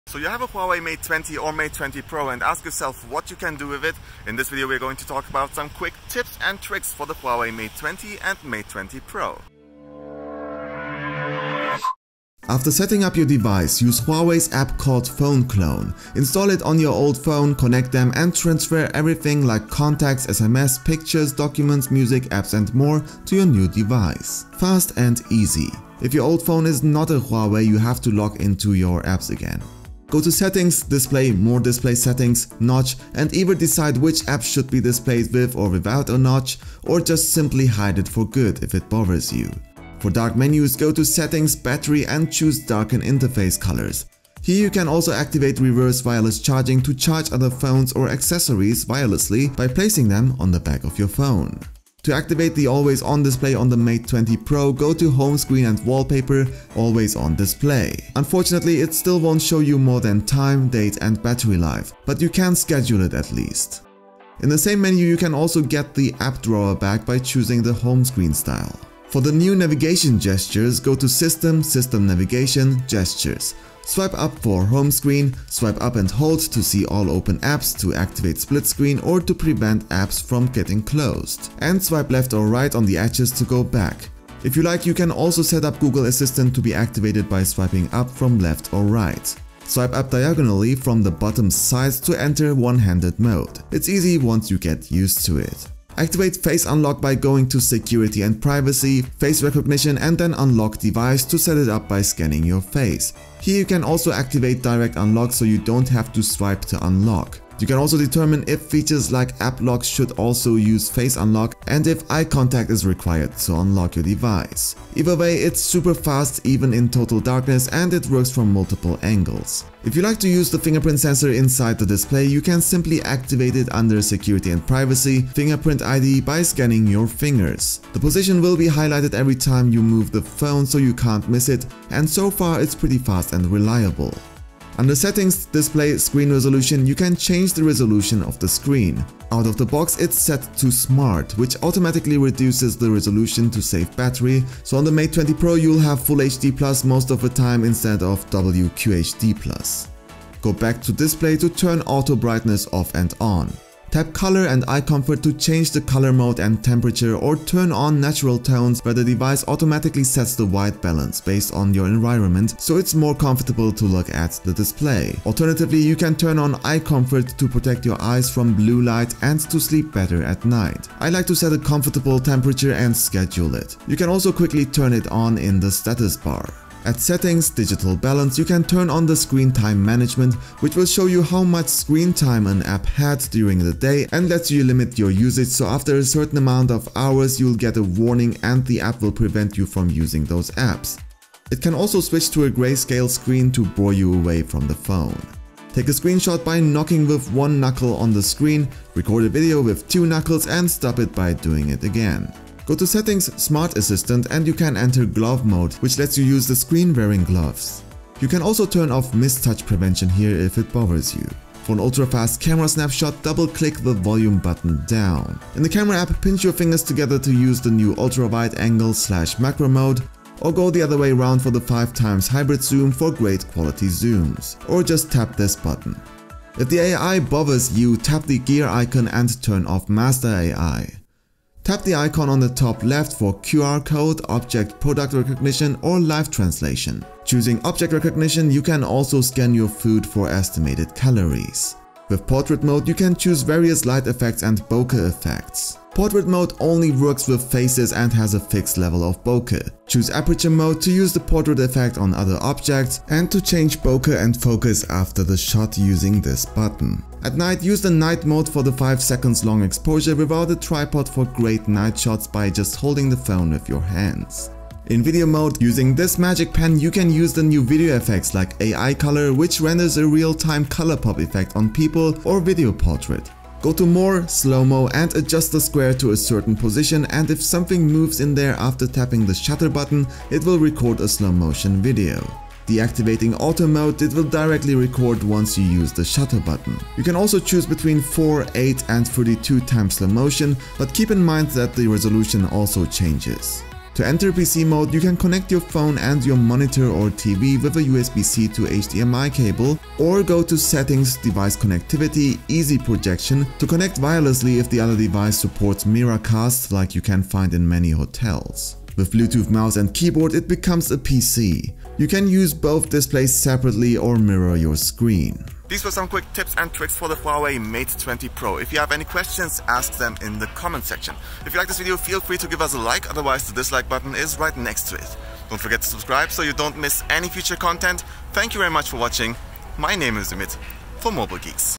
Huawei Mate 20 / Mate 20 Pro - 20 Tips and Tricks (EMUI 9 / Android 9)
The Huawei Mate 20 and Mate 20 Pro are the newest flaggships from Huawei. For a good start we have prepared 20 tips and tricks. If you have any questions, ask them in the comment section.
In this Video you´ll see:
- Transfer files and settings from the old to the new phone using Phone Clone
- Turn off the / deactivate the Notch
- Activate dark menus
- Reverse Wireless Charge other devices with the Mate 20 Pro
- Activate Always on Display
- Activate the App Drawer
- Activate the new Navigation gestures and learn how to use them
- Set up Face Unlock
- Set up the In Display Fingerprint sensor
- Change the display resolution
- Turn off auto brightness
- Activate Natural Tone (automatic white balance of the screen), change color mode and color toness, customize blue light filter (Eye Comfort)
- Take a screenshot by knocking
- Record the screen (video) by knocking
- Activate glove mode
- Deactivate mistouch prevention
- Take pictures super fast in 0.3 seconds from standby with Ultra Snapshot
- Use ultra wide angle camera, macro mode, hybrid zoom
- Deactivate Master AI
- Object recognition and measure calories with the camera
- Use Portrait Mode
- Use Aperture Mode
- Use Night Mode
- Use Video Effects (Classic Movie Color) like AI Color Pop and AI Portrait
- Use Slow Motion right
- Wireless PC Mode with Miracast